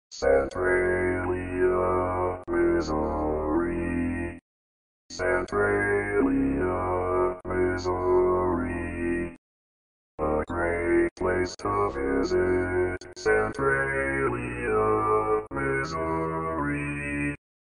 How to Say or Pronounce USA Cities — Centralia, Missouri
This video shows you how to say or pronounce Centralia, Missouri.

A computer said Centralia, Missouri. How would you say Centralia, Missouri?